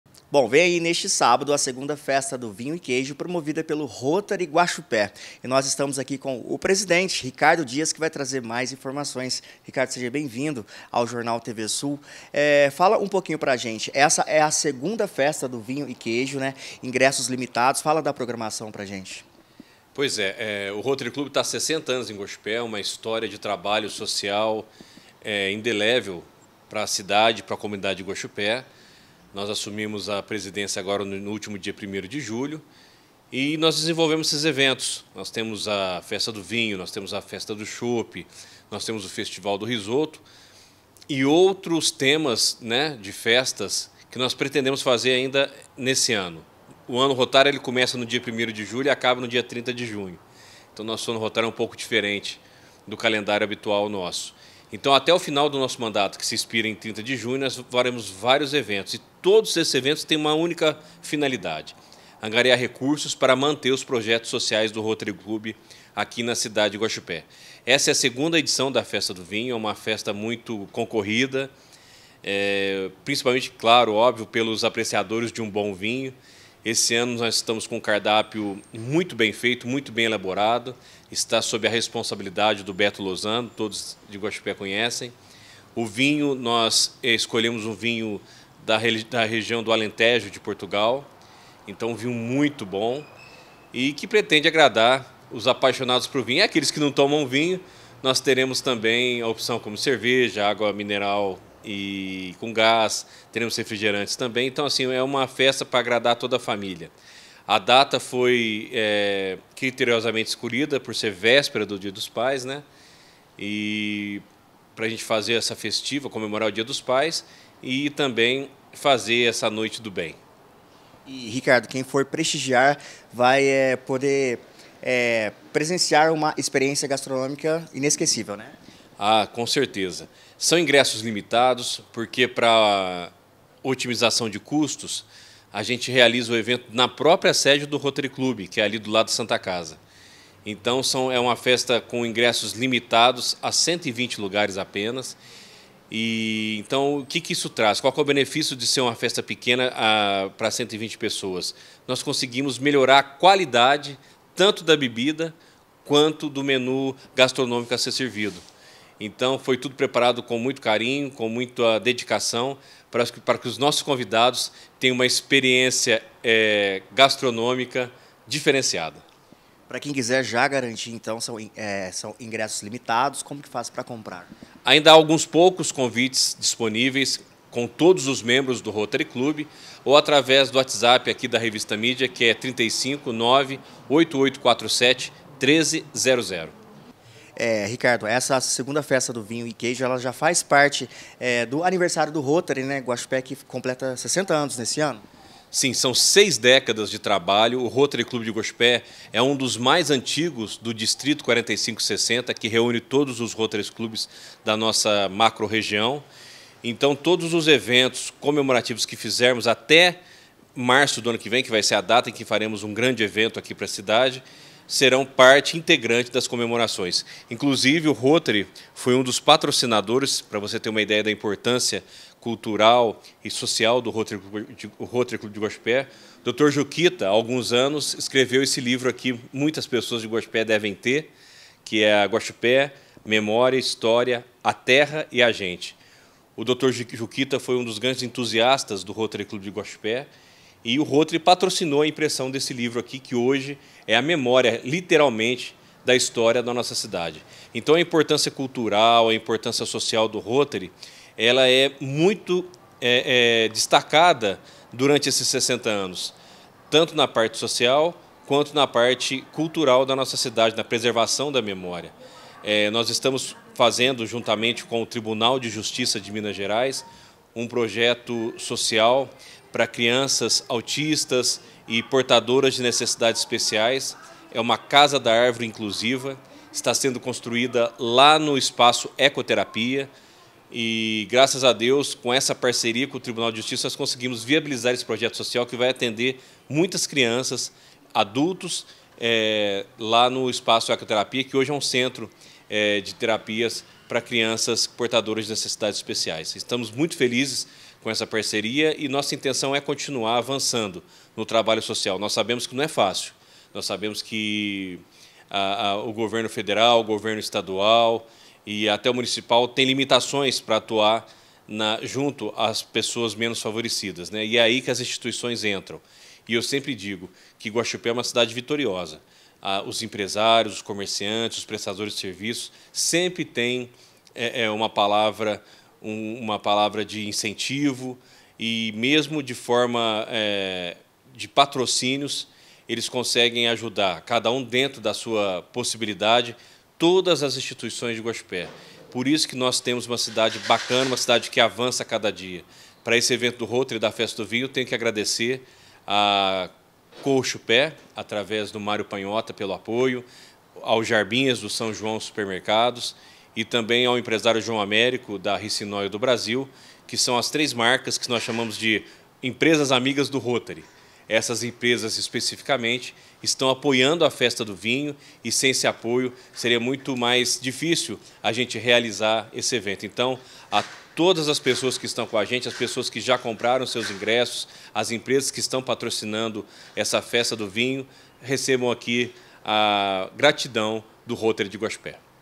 2ª Festa do Vinho e do Queijo do Rotary Club Guaxupé: uma noite do bem, de sabores e tradição
Neste sábado, 10 de agosto, o Rotary Club Guaxupé promove a 2ª Festa do Vinho e do Queijo. Na entrevista a seguir, o presidente do Rotary Club Guaxupé, Ricardo Dias, compartilha mais detalhes sobre o evento.